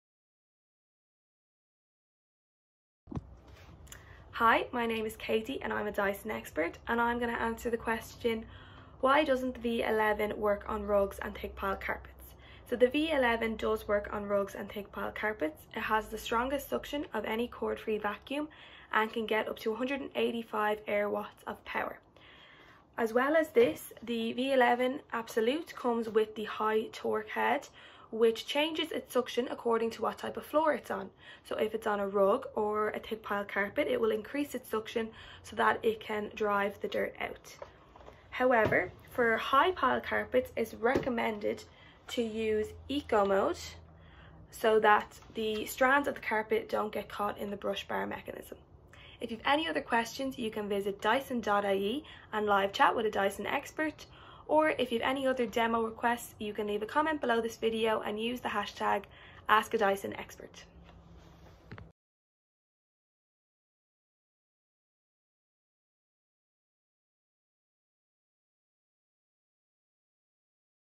Why doesn’t the Dyson V11™ work on rugs and thick pile carpets?
You asked us why the Dyson V11™ vacuum doesn't work on rugs and thick pile carpets. Dyson Expert Katie answers with this video she filmed from her home.